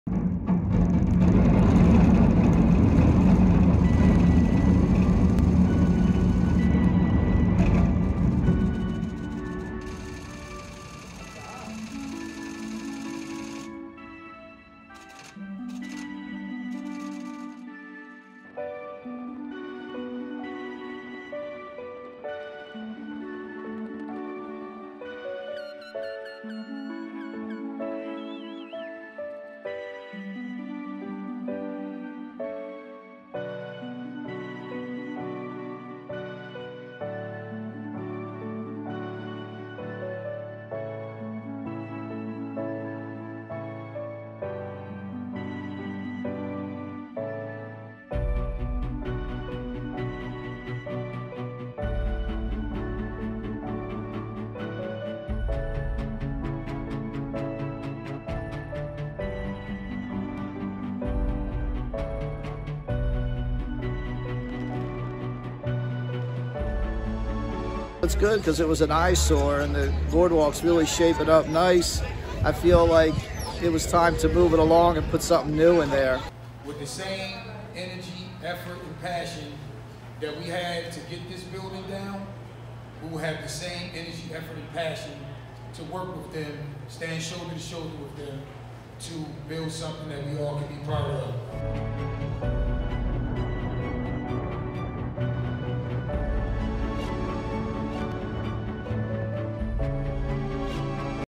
Trump Plaza implosion draws crowds in Atlantic City
Crowds gathered to watch the implosion of the building that housed the former Trump Plaza Casino in Atlantic City.